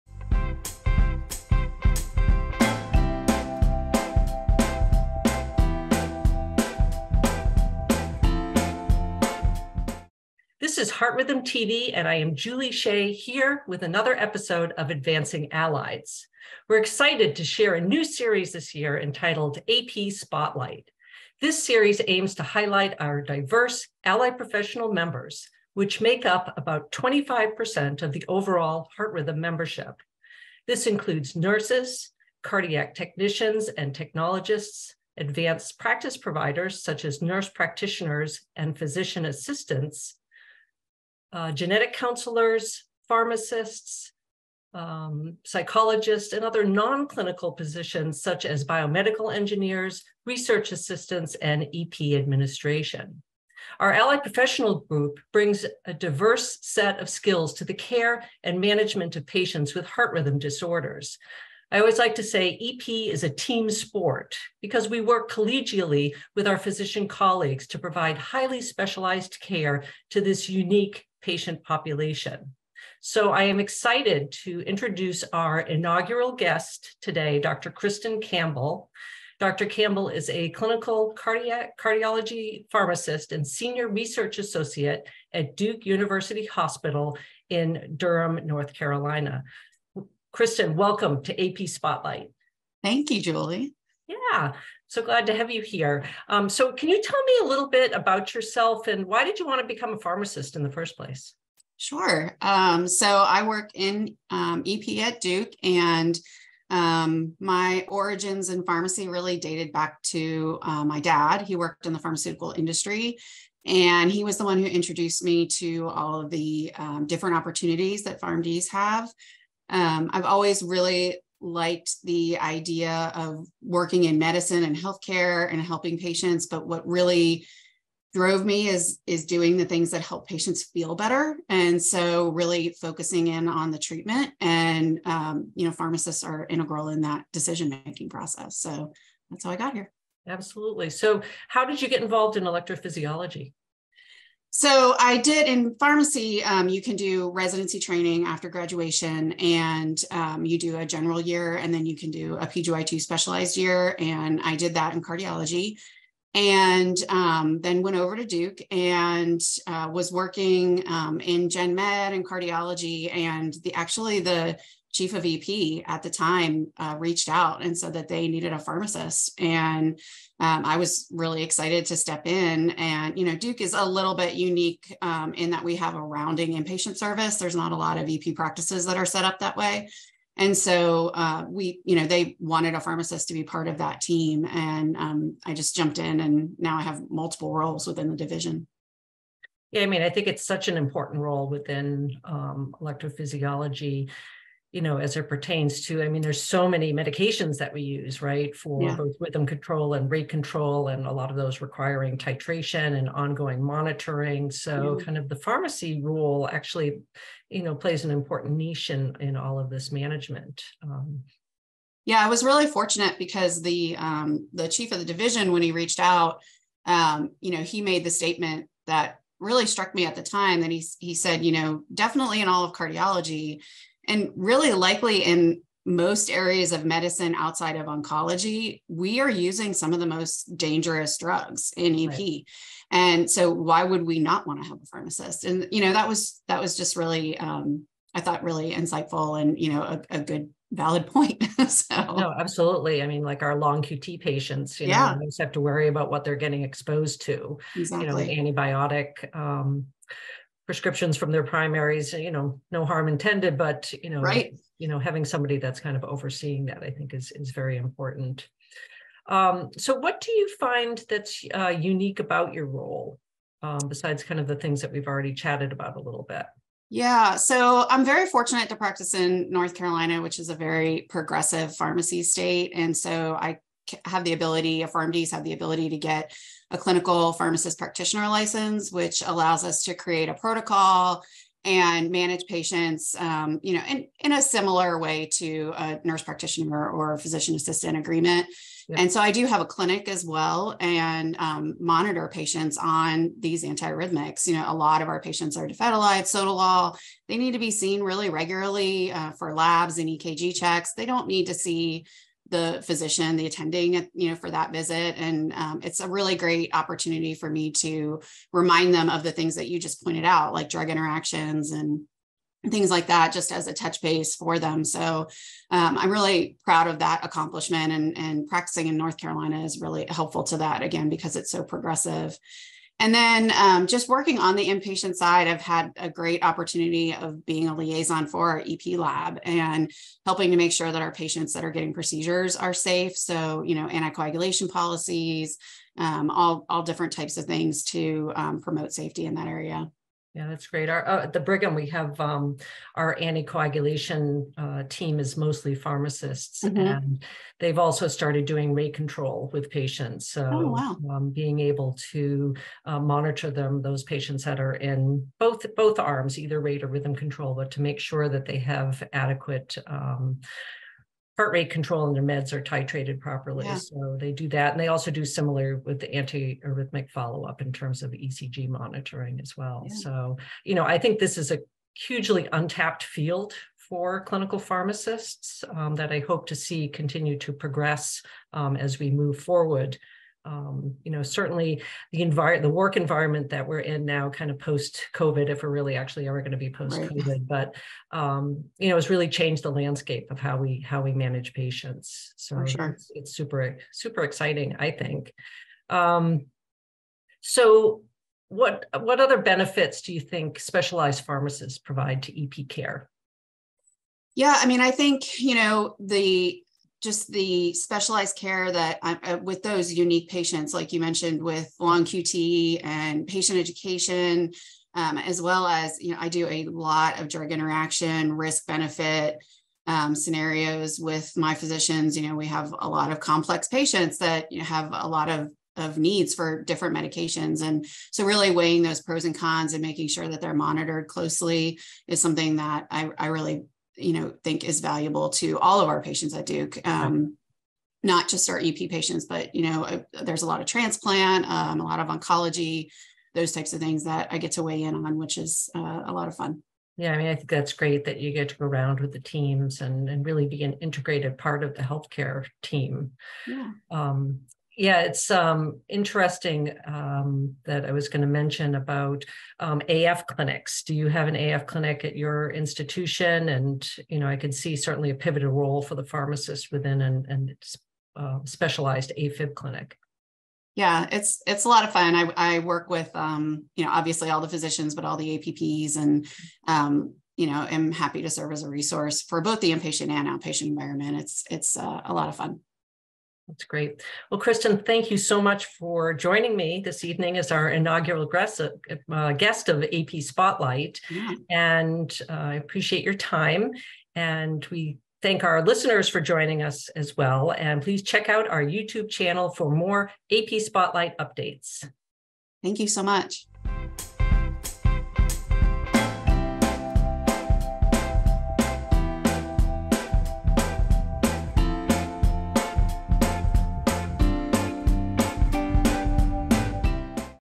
Advancing Allieds: AP Spotlight (Episode 1) - Kristen Campbell
In this "Advancing Allieds" episode, HRStv host Julie B. Shea, MS, MSN, FHRS, CCDS sits down with featured AP Spotlight guest, Kristen B. Campbell, PharmD. She discusses her unique role as a clinical pharmacist working in the field of Cardiac Electrophysiology, detailing the balance of in-patient and outpatient care as well as the management of patients on various antiarrhythmic regimens. Specific benefits of dedicated pharmacist care are discussed as well as the future of this role in EP.

J Am Coll Cardiol. 2015 Nov 10;66(19):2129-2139. doi: 10.1016/j.jacc.2015.09.025.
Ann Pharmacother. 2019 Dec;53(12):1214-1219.